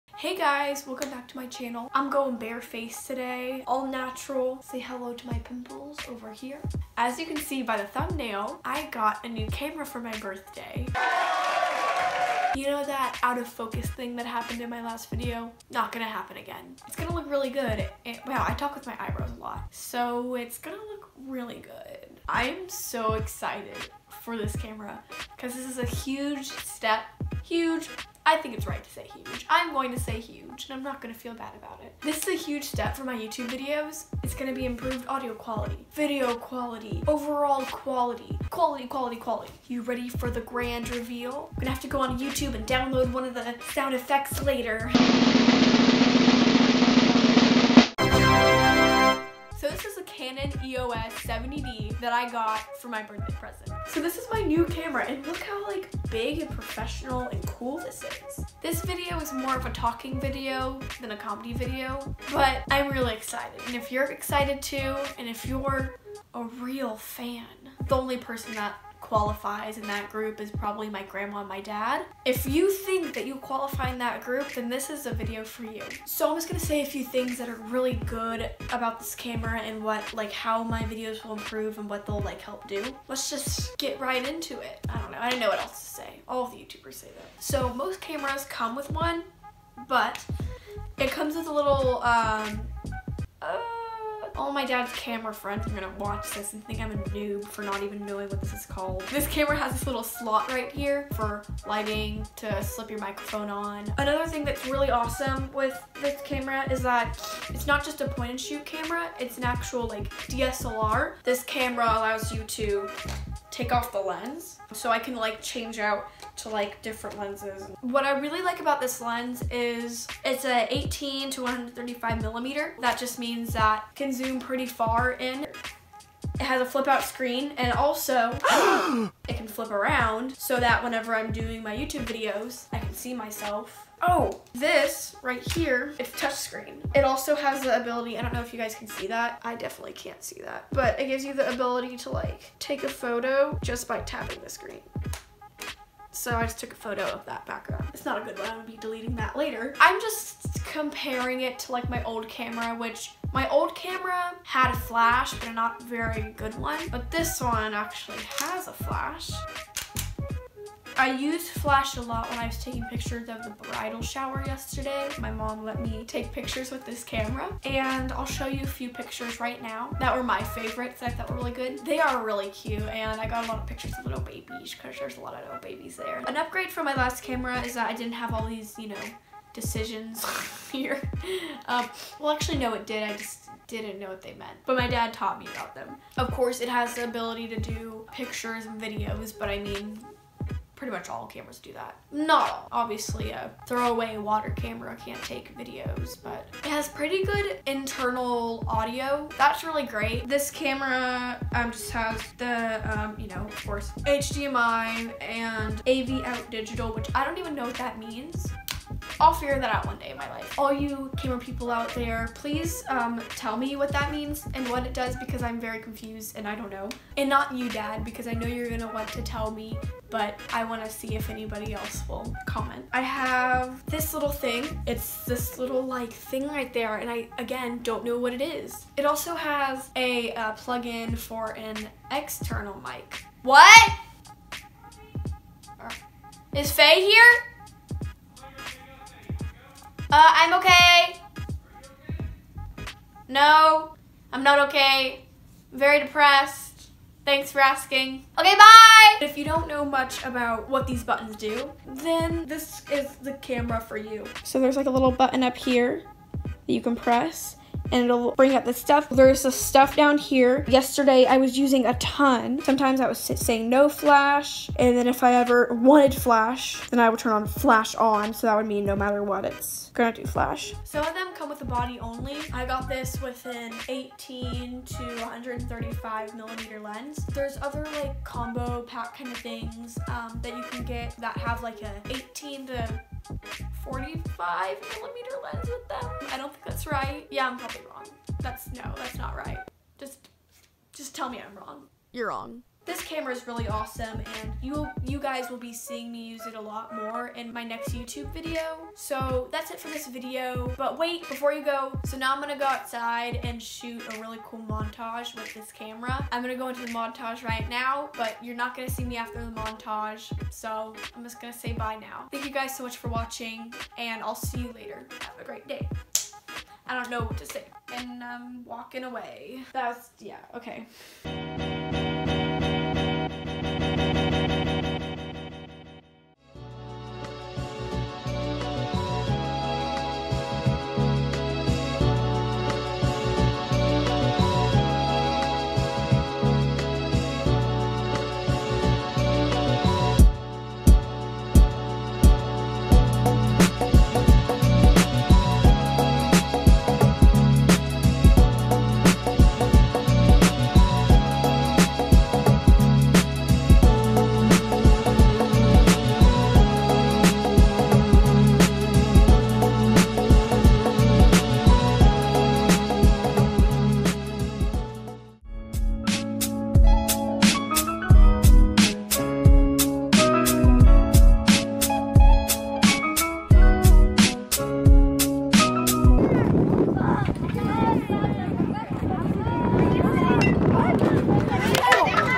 My New Camera! // Canon EOS 70D
I got a new camera! I'm making a video about this because this camera is a huge step for my YouTube channel. I'm so excited to start using it for my videos. I used to just use my phone for my videos, but now I get to use an actual camera! Hopefully, you guys notice as big of a difference as I did. But anyways, hope you enjoyed and have a great day!